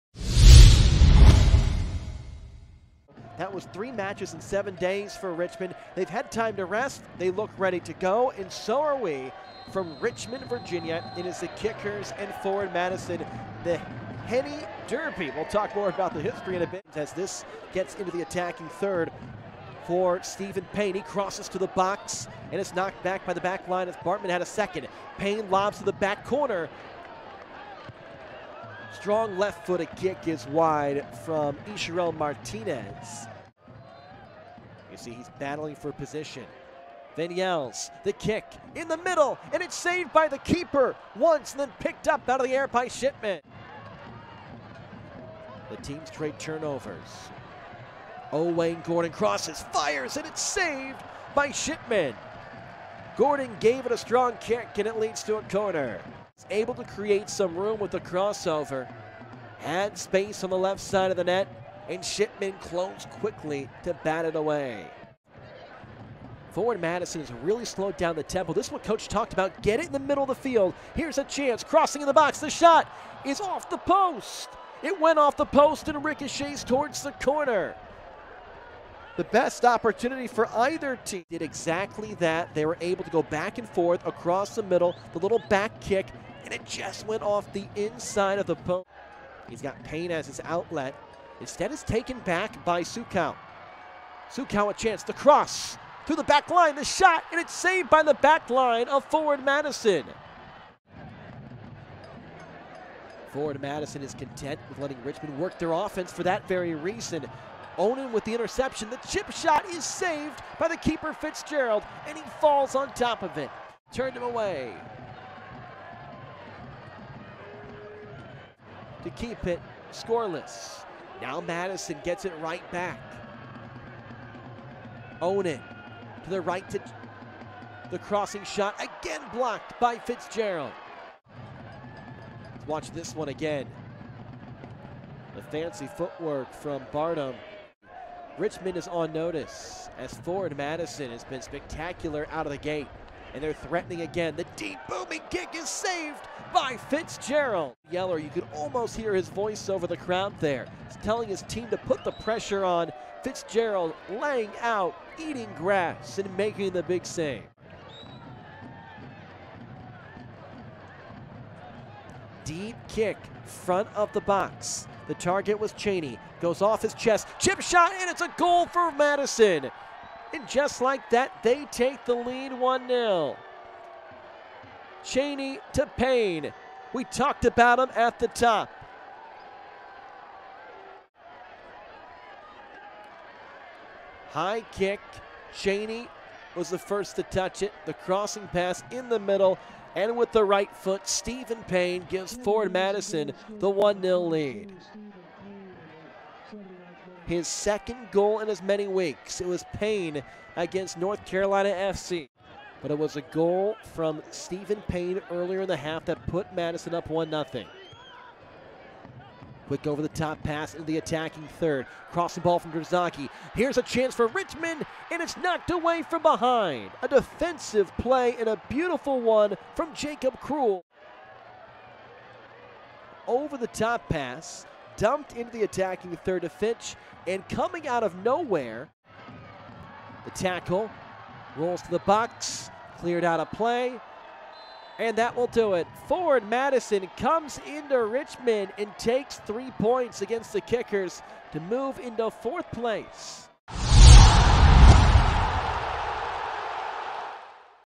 6.17.2023 | Richmond Kickers vs. Forward Madison FC - Game Highlights
Stephen Payne breaks the draw late against his former squad to lift Forward Madison FC over the Richmond Kickers in the 12th all-time Henny Derby.